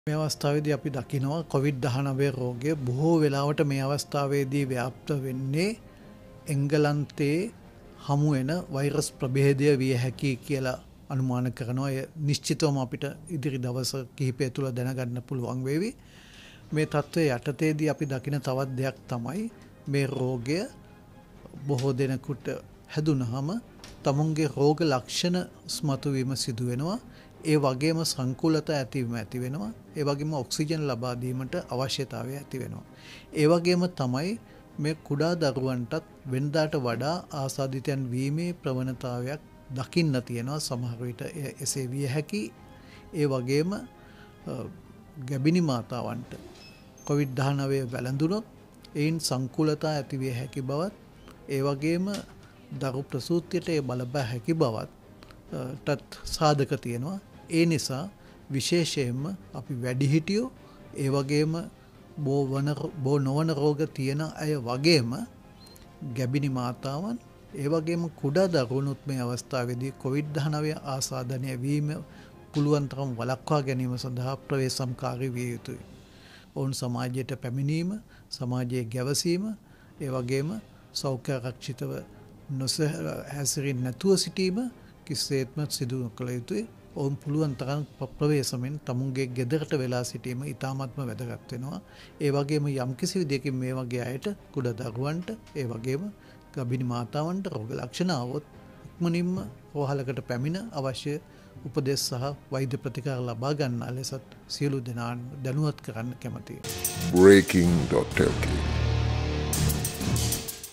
කුඩා දරුවන්ටත් මේ කොරෝනා ප්‍රබේදය වැලදීමේ ප්‍රවනතාවය වැඩි | Breaking.lk
කුඩා දරුවන්ටත් මේ කොරෝනා ප්‍රබේදය වැලදීමේ ප්‍රවනතාවය වැඩි